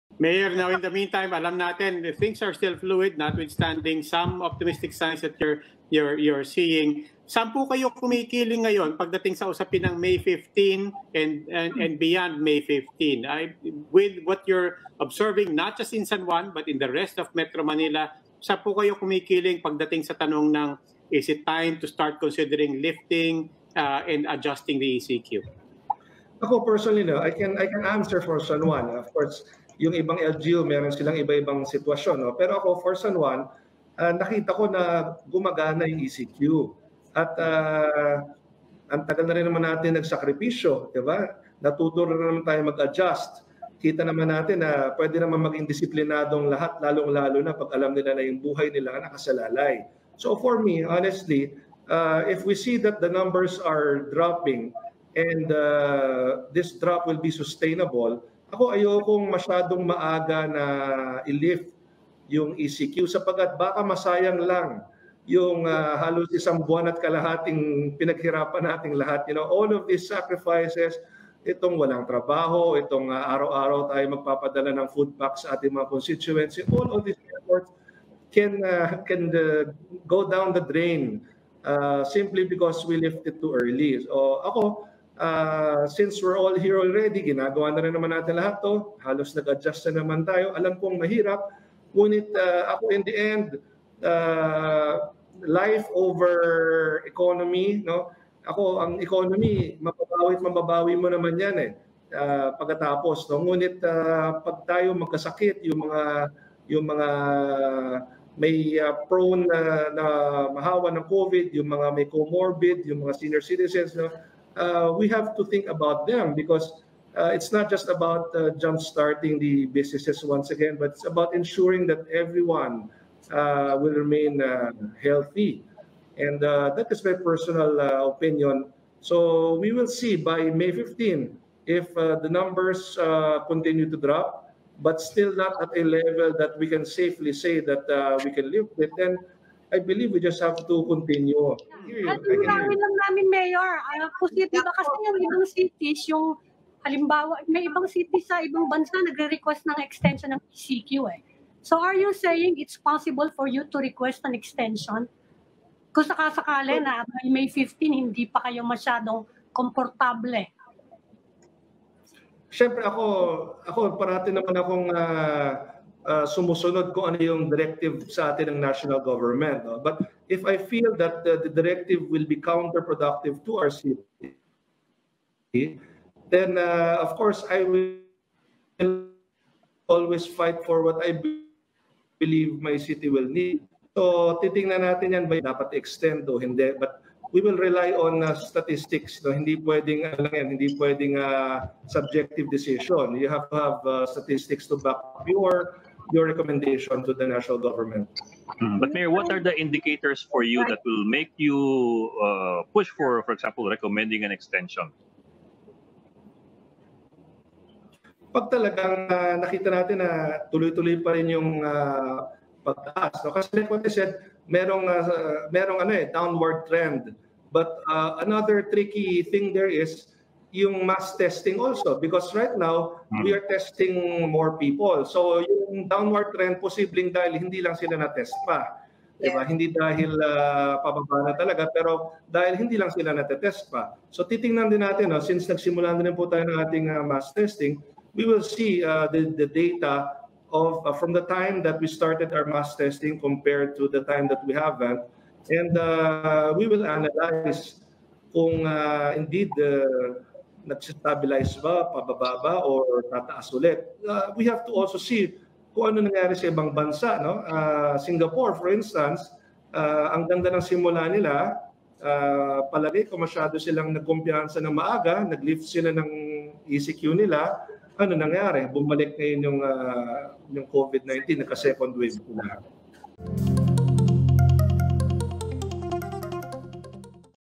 San Juan Mayor Zamora not keen on lifting ECQ by May 15
TheChiefs | San Juan City Mayor Francis Zamora says he wouldn't support an 'early lifting' scenario of the Enhanced Community Quarantine after May 15 even if trends by then show a sustained drop of COVID19 cases.